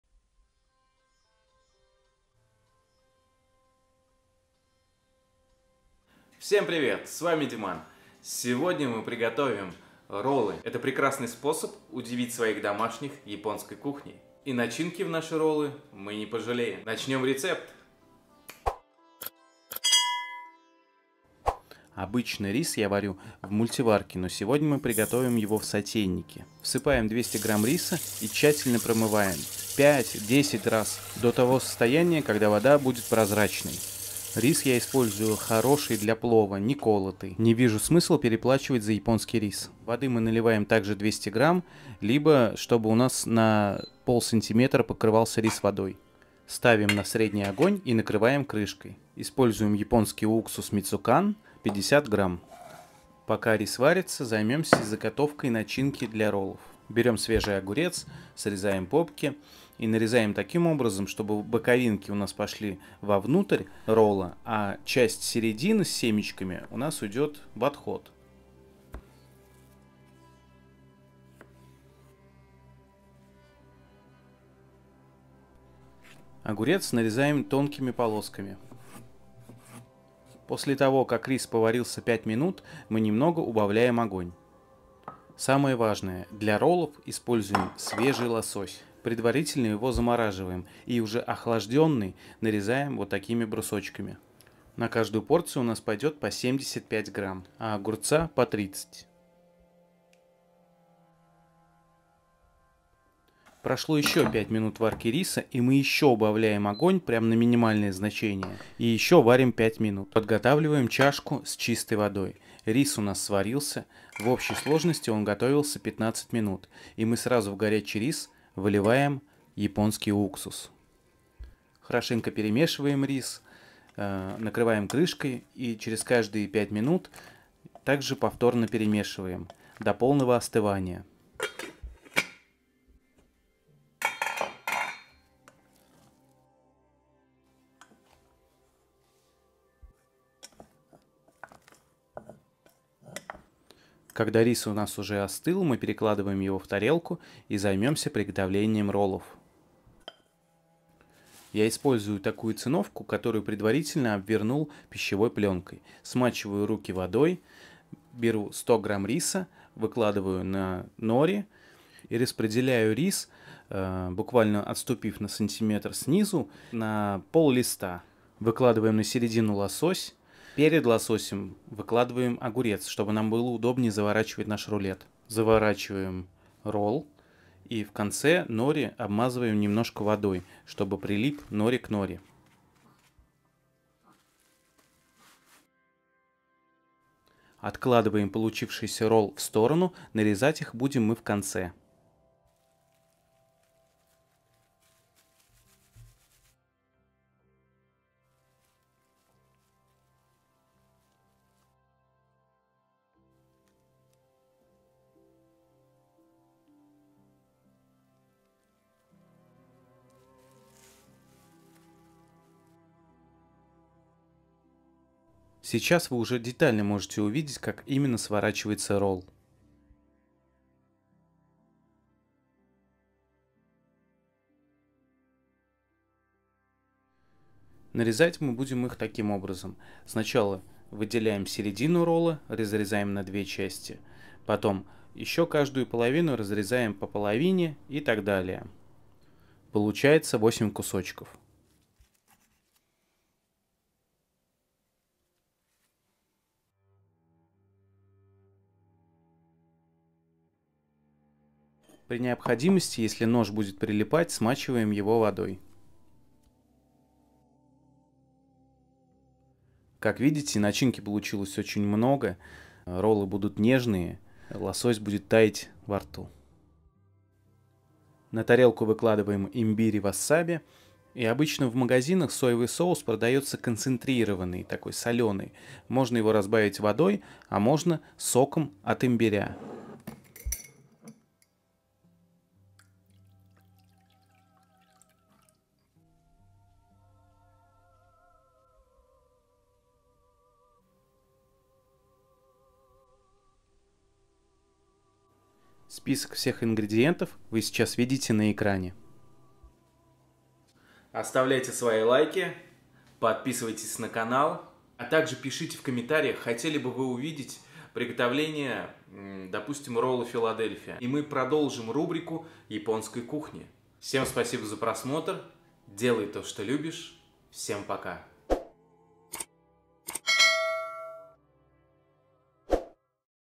Роллы в домашних условиях. Как приготовить РОЛЛЫ дома.
Сегодня мы приготовим настоящие роллы дома!

Ингредиенты:
Рис 200гр (не обязательно именно японский, самое главное хорошего качества)
Уксус мицукан 50гр
Нори 4 листа
Огурец 120гр
Свежий лосось 300гр
Имбирь васаби соевый соус

Для начала промываем рис в холодной воде 5-10раз до прозрачного цвета воды. Далее оставляем воды в сатейнике 1/1 с рисом что б вода чуть прикрывала рис. Ставим варится на среднем огне на 15мин чуть уменьшая огонь через каждые 5 мин. Далее сразу вливаем уксус в рис и перемешиваем каждые 5 мин рис до полного остывания.

Огурец нарезаем соломкой вырезав сердцевину (она нам не понадобиться)
Лосось (не соленый) (можно предварительно заморозить) нарезаем толстыми брусочками.

На лист нори выкладываем рис и распределяем равномерно по листу нори примерно на 1/2 листа.
Выкладываем на рис лосось и огурец и сворачиваем рулетом ролл. Нарезаем на 8 кусочков.

😋 приятного аппетита

Поддержите ЛАЙКОМ, КОММЕНТАРИЕМ и ПОДЕЛИТЕСЬ С ДРУЗЬЯМИ!